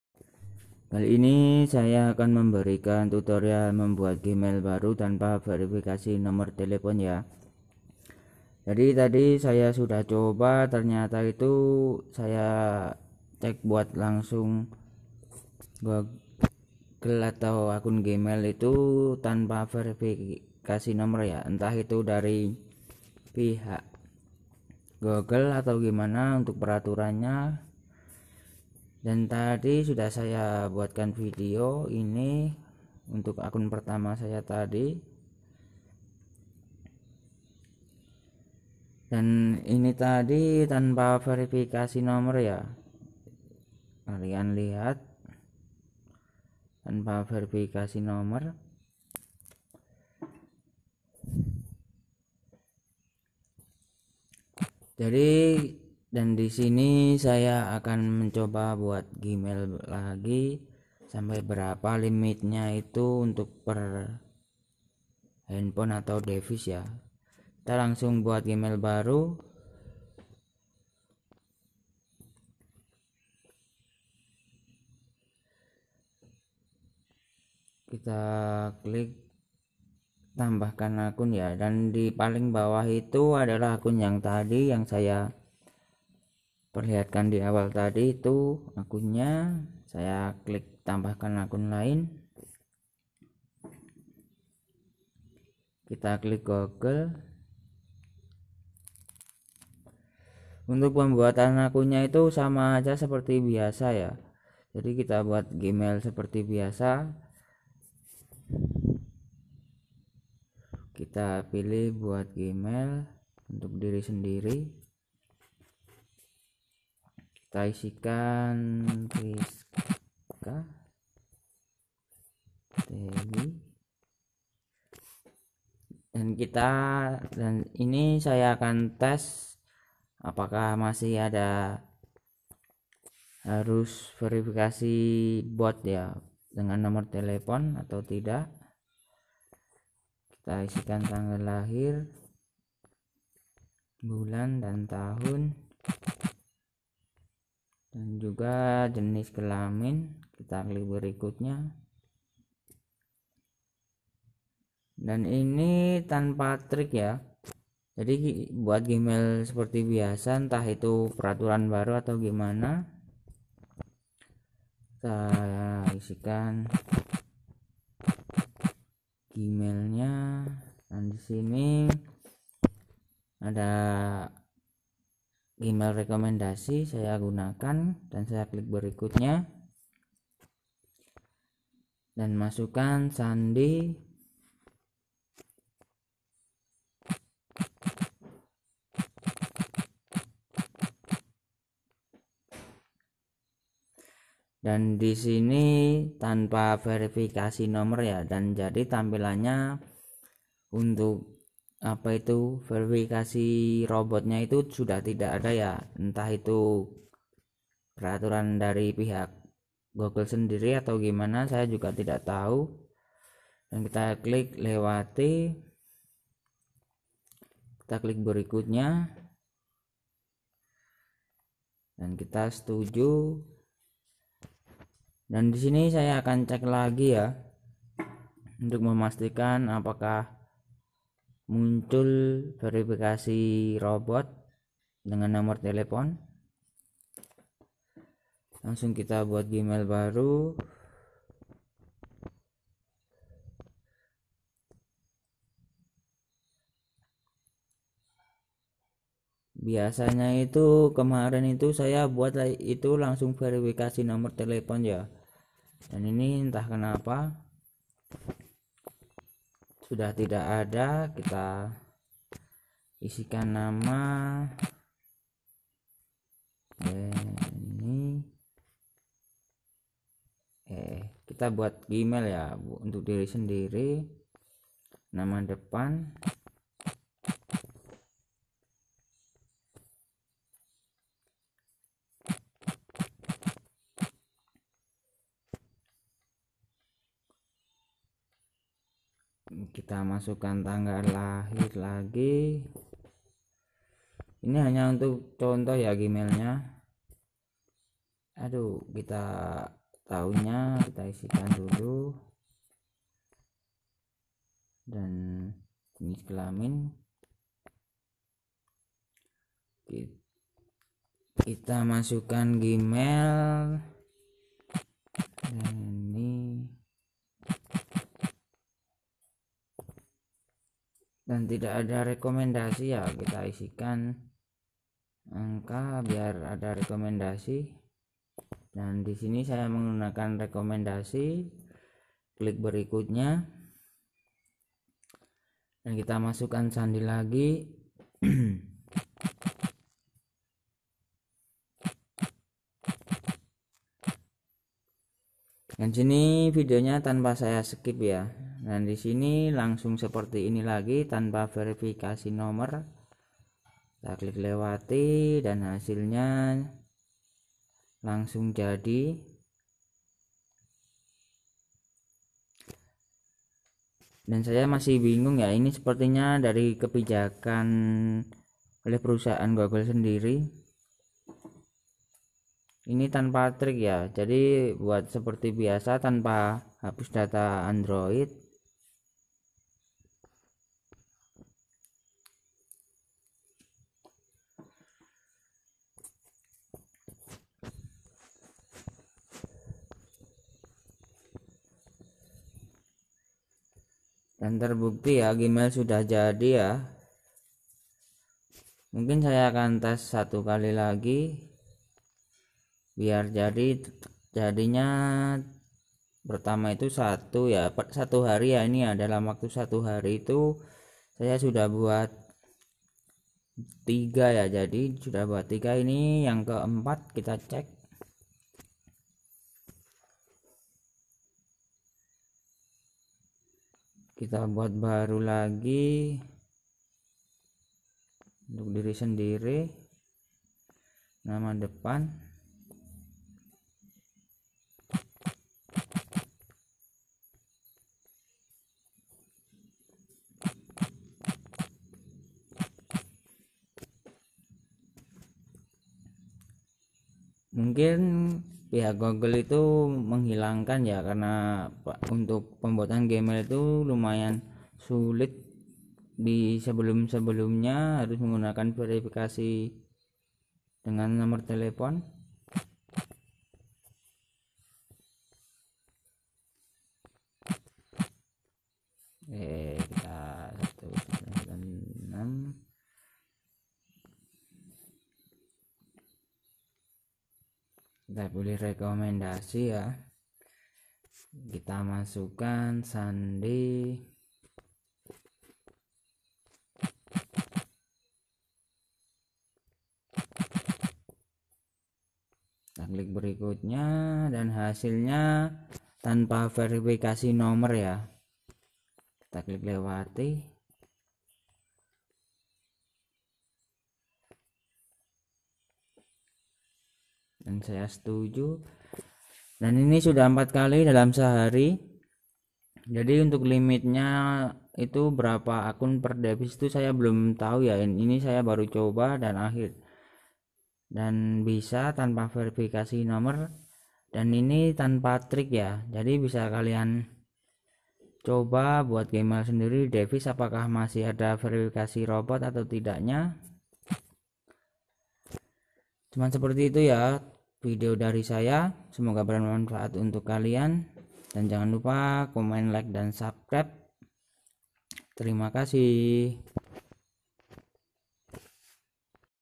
Membuat Akun Gmail Tanpa verifikasi 2021
Sepertinya pihak google memberikan peraturan baru untuk pembuatan akun gmail ,d peraturan ini kita membuat gmail tanpa menggunakan verifikasi nomer telepon/verifikasi robot .
namun pembuatan gmail baru ini di batasi oleh pihak google ,
karna saya memcoba membuat 4 kali gmail dan di akun 4 dan seterusnya langsung di non aktifkan dan harus memverifikasi diri dan meminta peninjauan